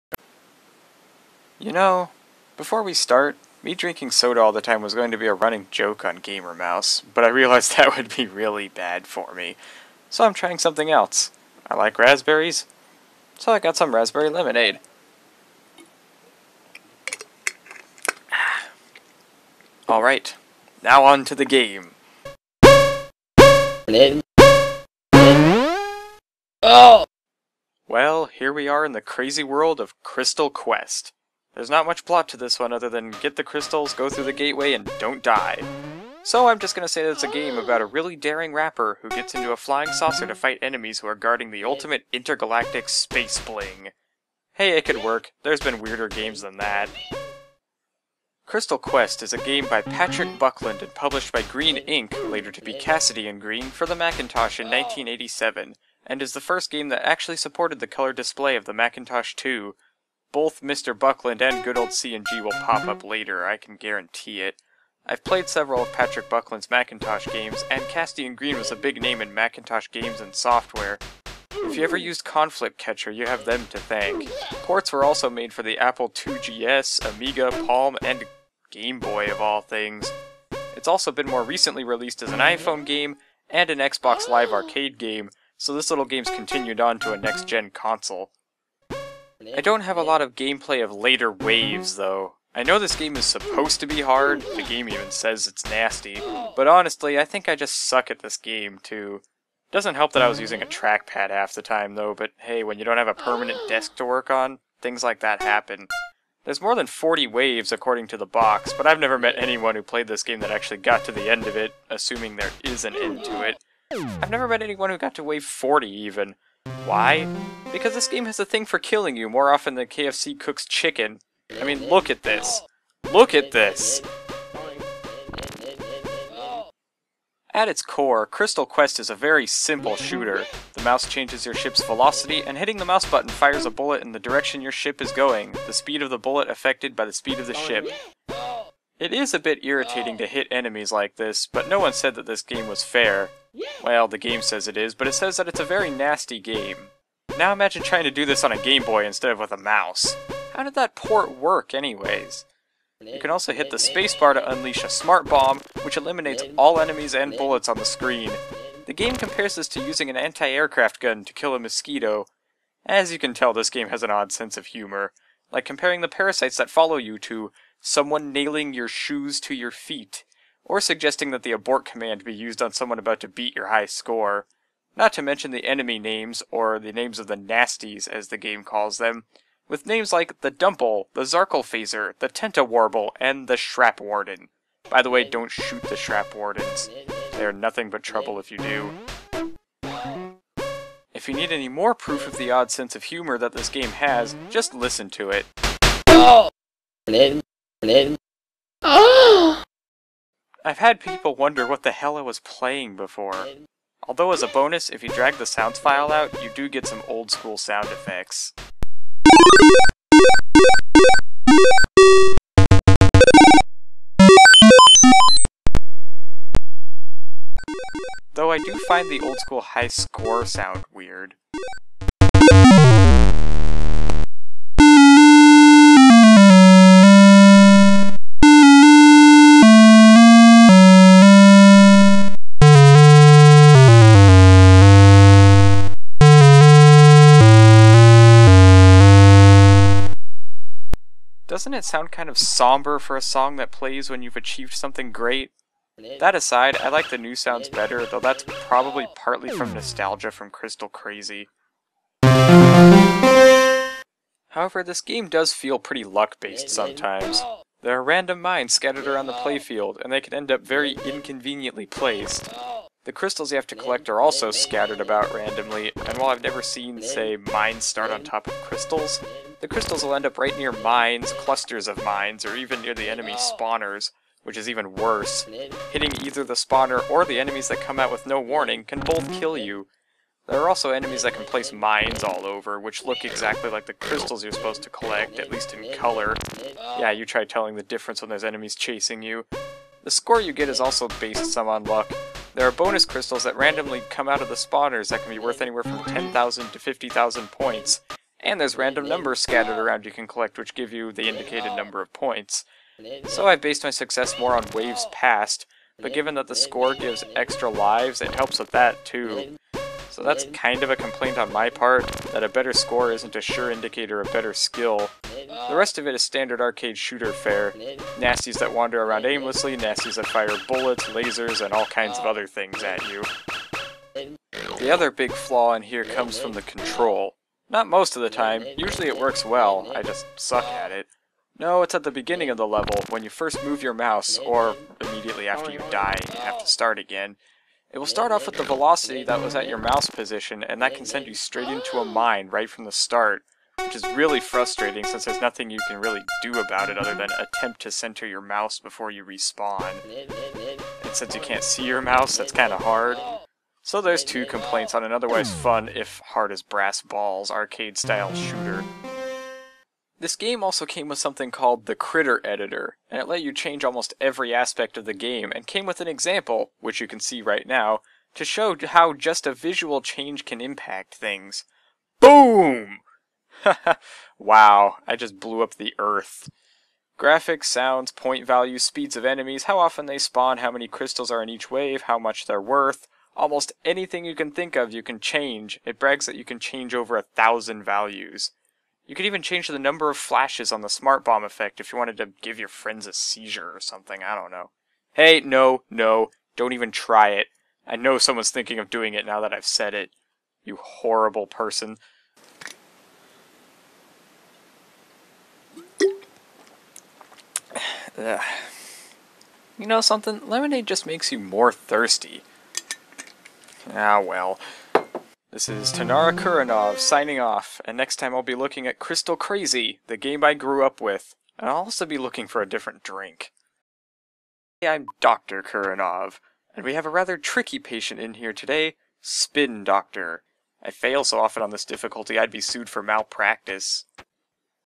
Gamer Mouse - Crystal Quest Review - Macintosh
Tanara Kuranov attempts to collect intergalactic space bling and shares his thoughts on lemonade in his review of Crystal Quest.

Wow, thanks, YouTube. Give me three thumbnails that are exactly the same damn thing. Classy.

Also, why do I keep getting games with lots of red (looking at you, Vette) and blue against black? It's horrible in H.264. Damn chroma subsampling, when do we get the option to not have that?

Crystal Quest belongs to Patrick Buckland and Casady & Greene, 1987